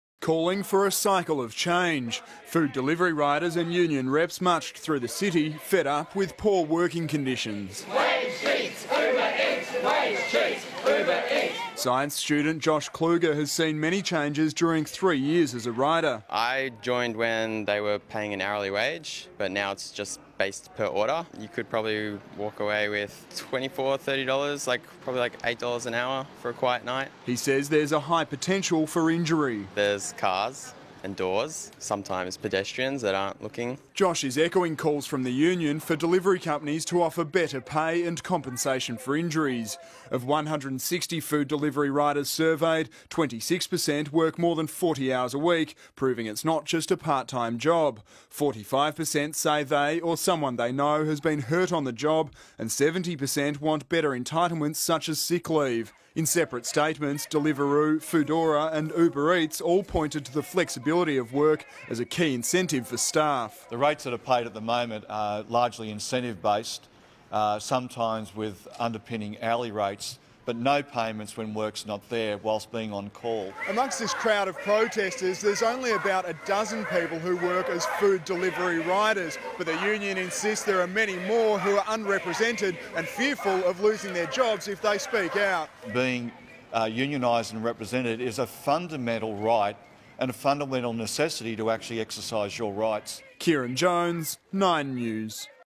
Food delivery riders protest poor pay and a lack of compensation
( 9 News, 31 Jan 2018) Food delivery riders have taken to the streets to protest poor pay and a lack of compensation.